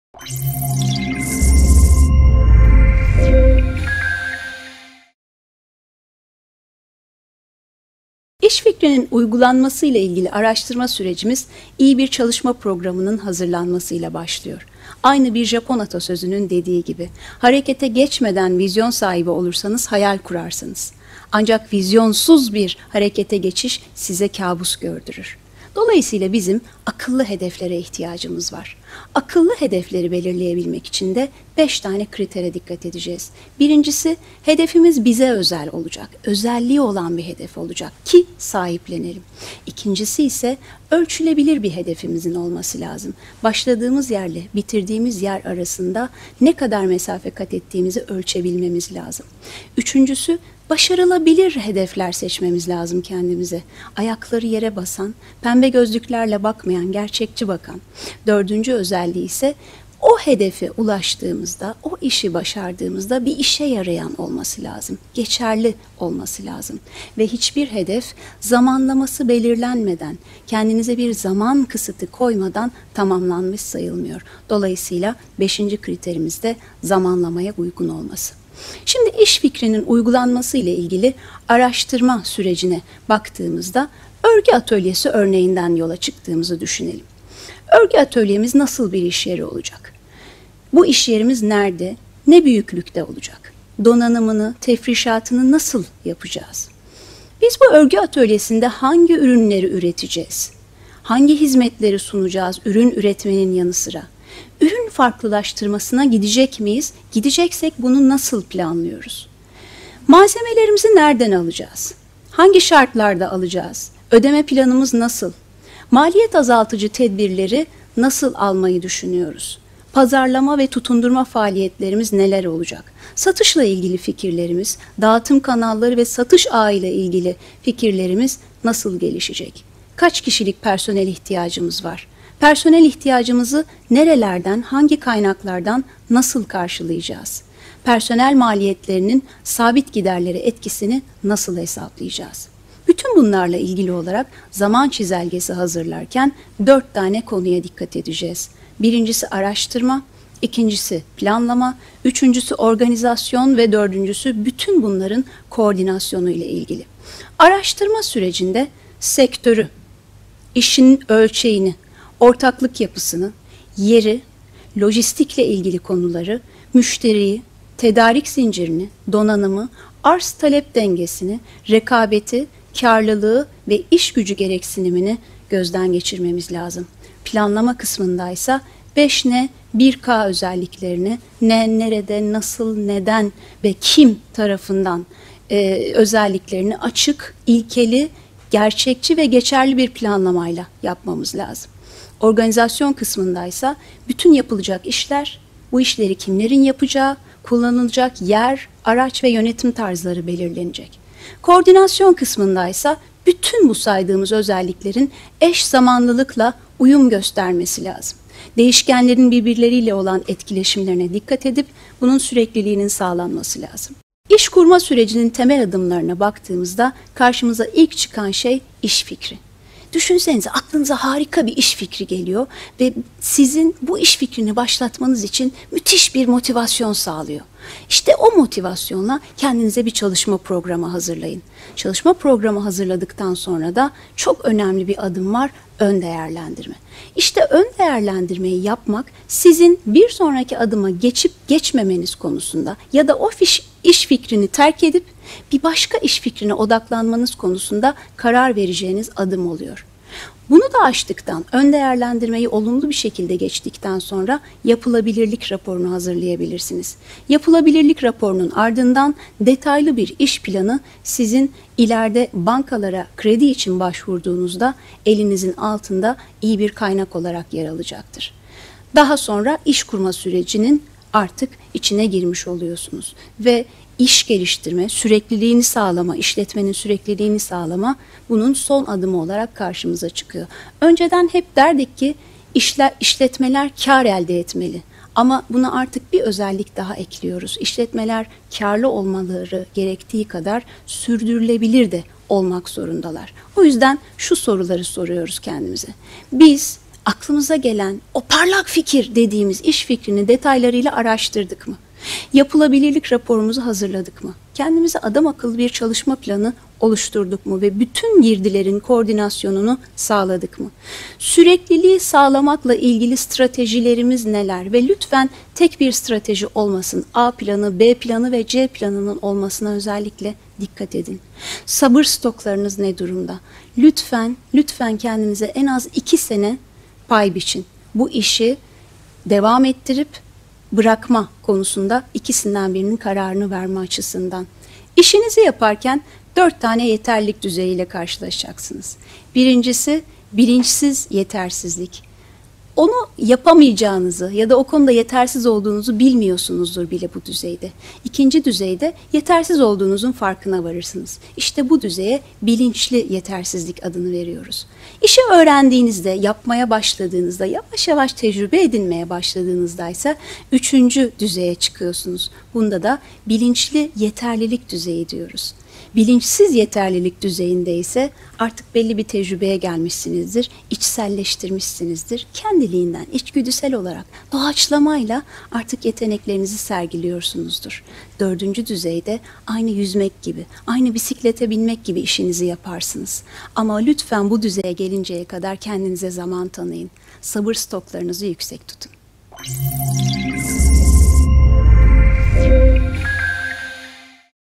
Kadın Girişimciler Rehberi: -4- Çalışma Programı Nasıl Hazırlanır
İş kurma sürecinde etkin bir çalışma programı hazırlamakla ilgili aydınlatıcı bilgiler…

Girişimcilikte vizyon sahibi olmak sürecin en önemli şartlarından biri…Bu eğitimde, “Smart”-Akıllı hedef özelliklerini, zaman çizelgesinin nasıl hazırlandığını, iş kurma sürecinin temel adımlarını ve işinizi yaparken yetkinlik düzeylerini anlatıyoruz. İş fikrinin uygulanmasıyla ilgili aklınızda oluşabilecek soruları cevaplıyoruz.